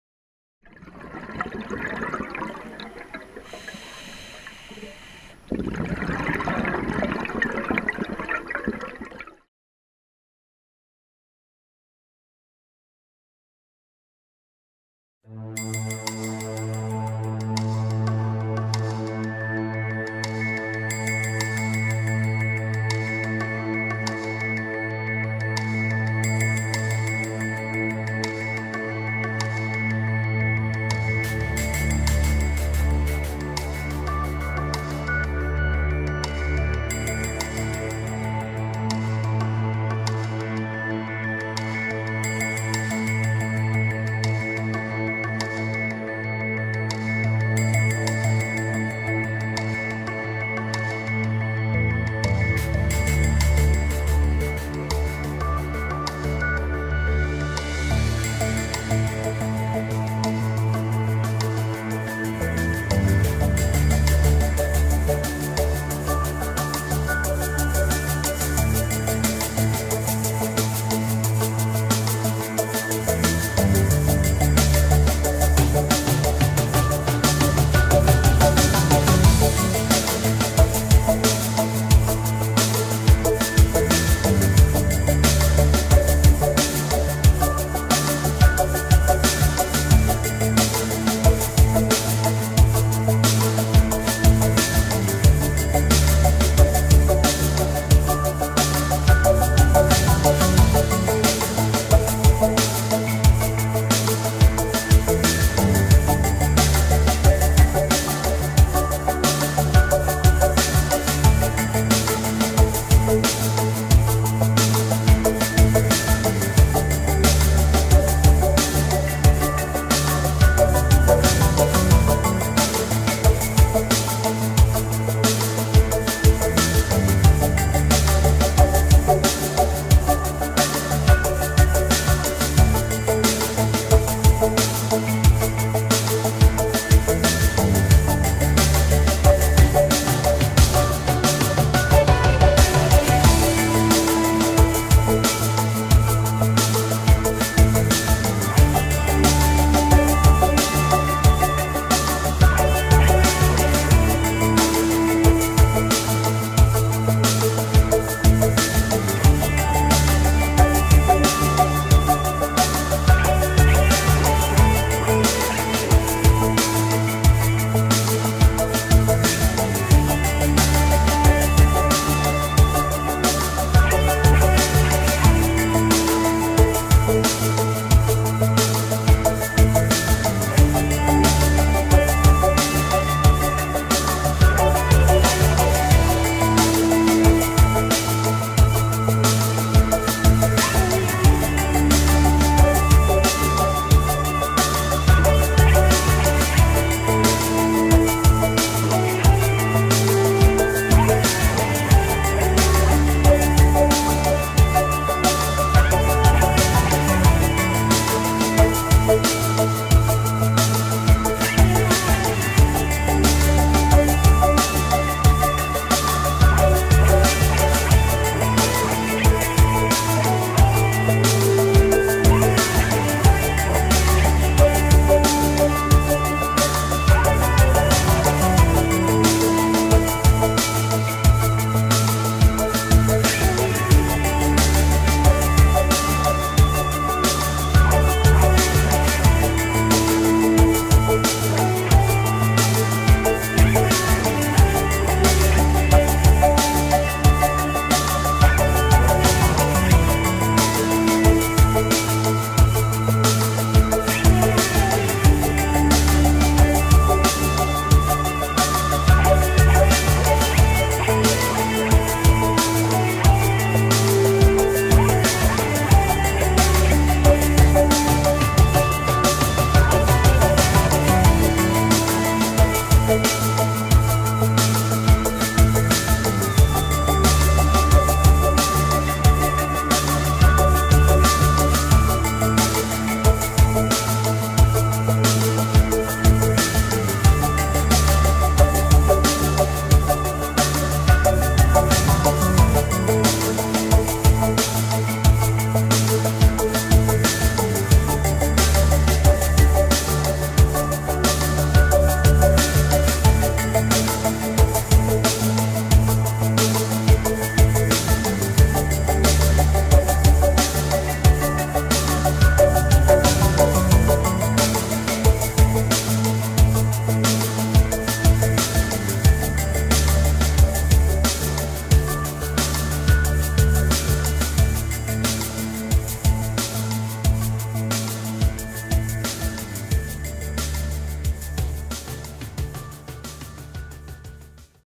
Diving Angaga Thila AKA Bobana Thila (HD) - South Ari Atoll, Maldives
Bobana Thila or also known as Angaga Thila is a typical and mindblowing reef in the heart of the South Ari Atoll, Maldives. Very good to spot turtles, stonefish, whitetip reef sharks and much more...

a 2015 H2O Explorers production